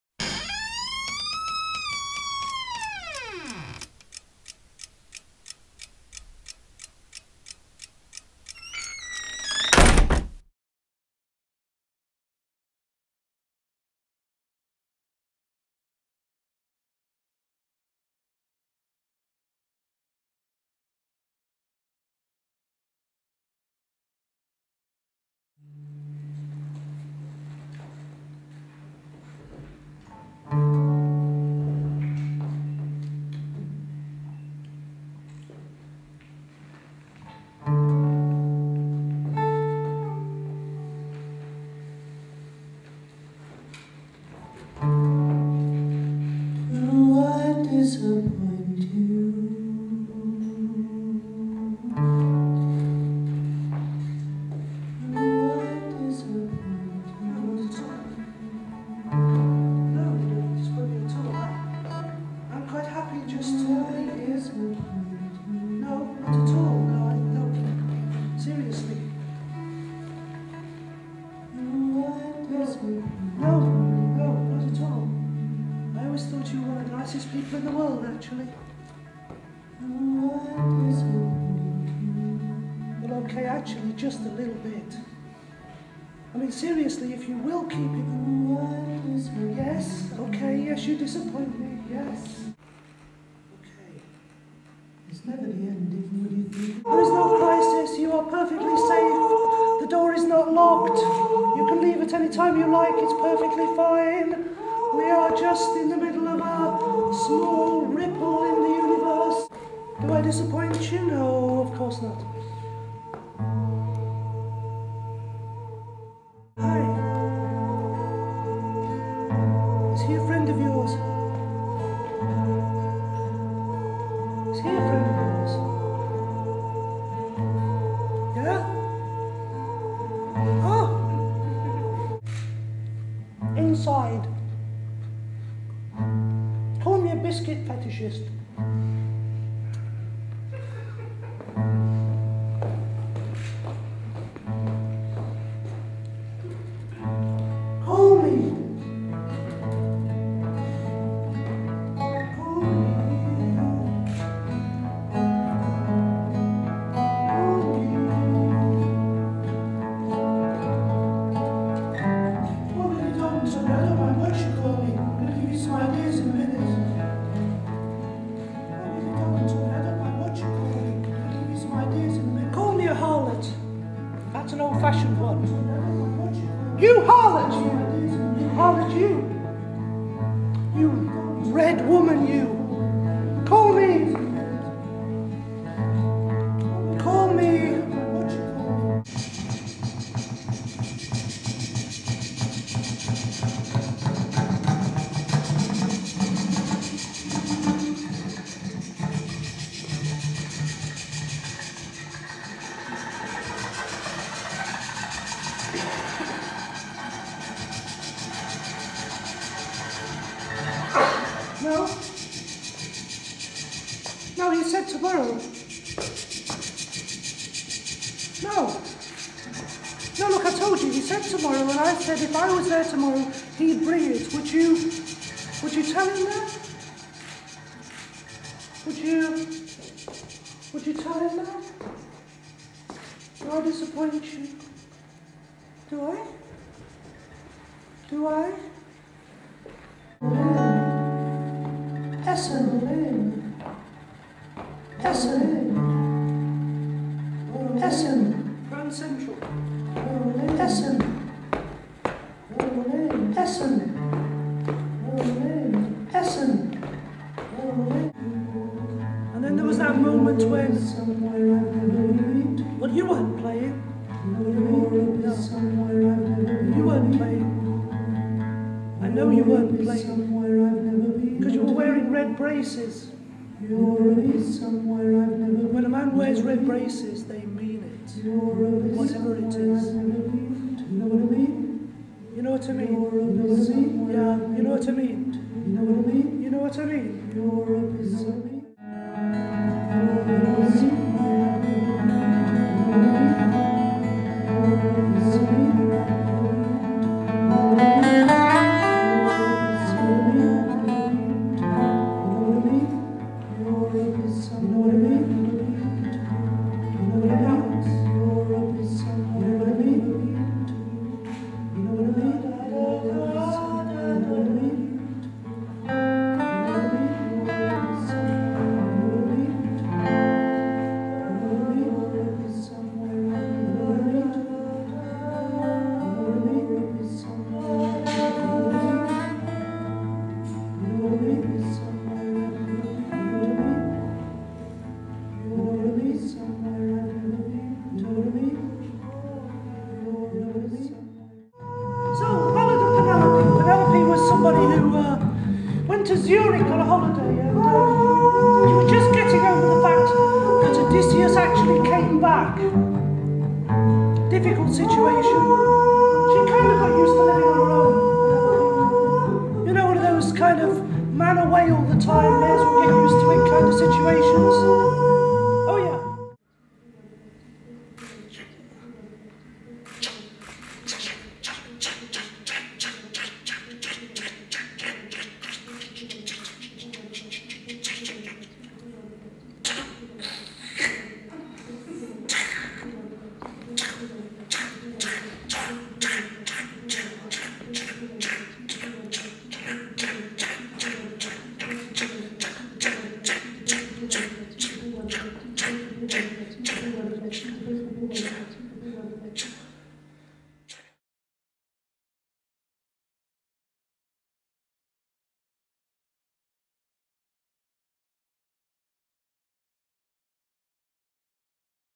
Performance MANDY ROMERO Exchange! Congress Berlin
Performance by Mandy Romero and Alex Decoupigny at the EXCHANGE! Congress for the Exchange Radical Moments! Live Art Festival.